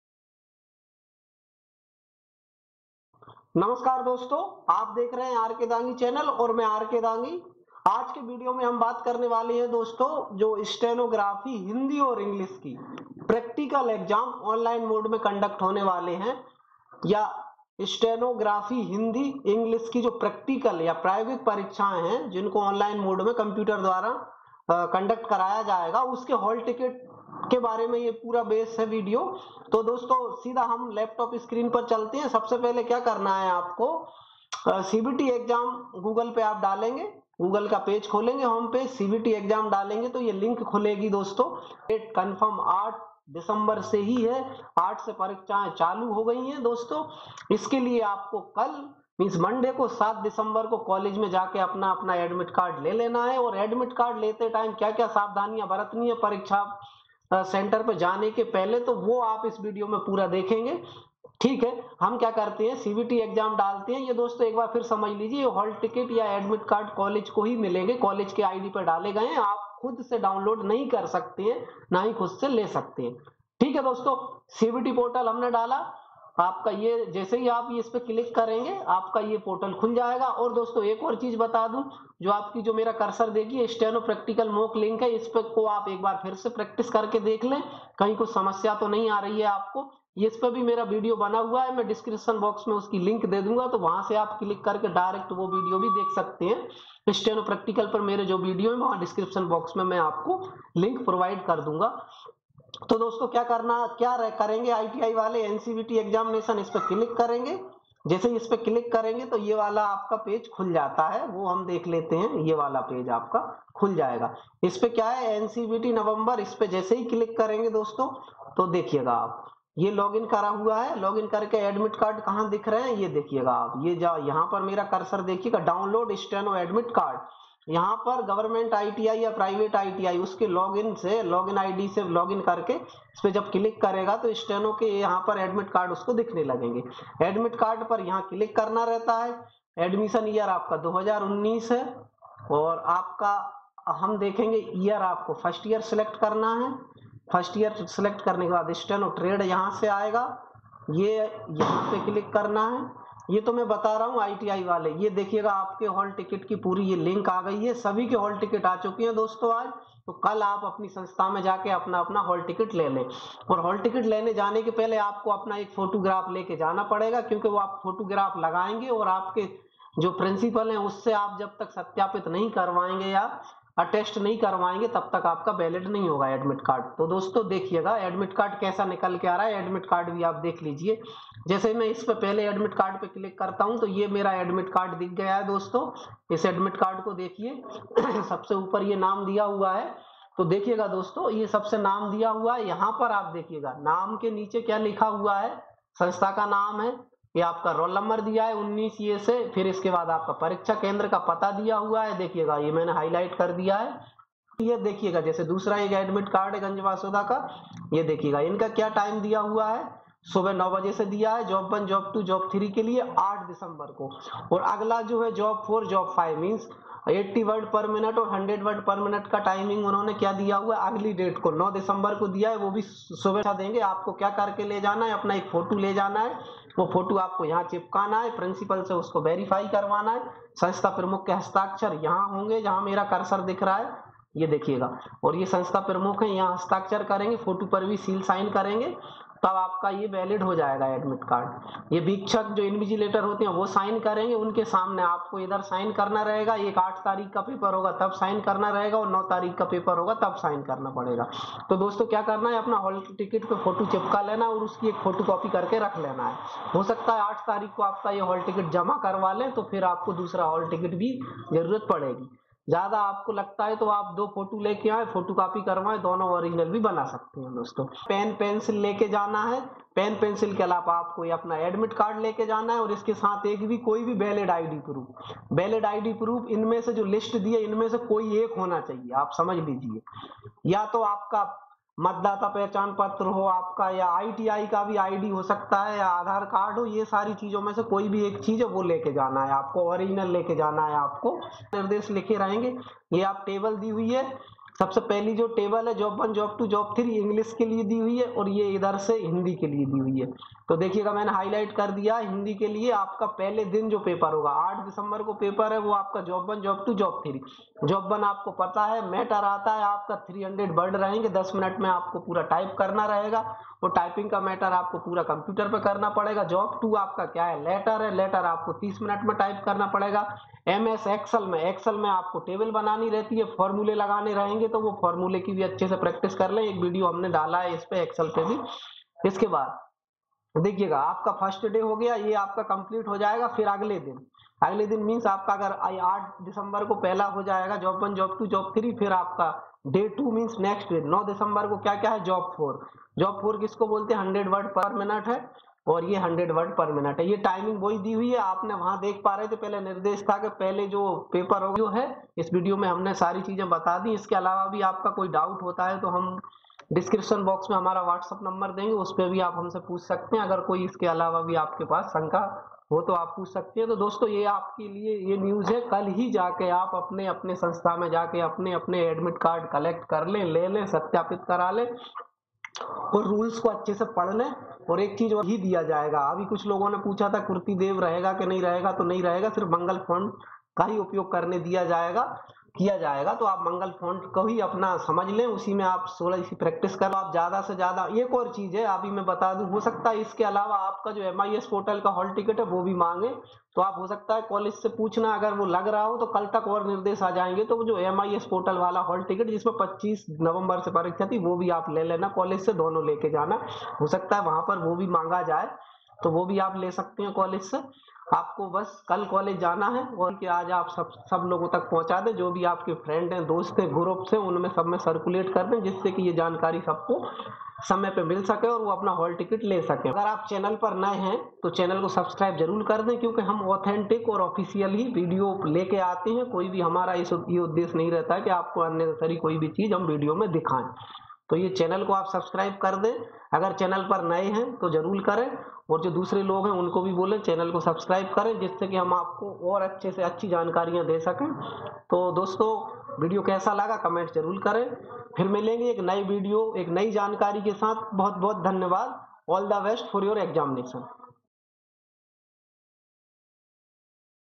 MP ITI Steno Hindi / English Practical Exam 2020 Admit Card is Out | ITI Practical Exam UserGuide
This video contains all about Steno HINDI and Steno English practical exam in online modes admit card 2020
 Rules for job1 , job 2, job3 and 80wpm dictation and steno
We give detail information of 100wpm practical exam
We give detail information of  practical exam Dec 2020 schedule
Tips for Excel formulas and table properties and job1 (matter )

PAPER 1  Online exam : job1, job2 and jobs 3
PAPER 2  Online exam : job 4 (steno 80wpm) and job5 (Steno 100wpm)

Video link for steno practical exam 2020 given below
Watch these video please:

WhatsApp group for govt jobs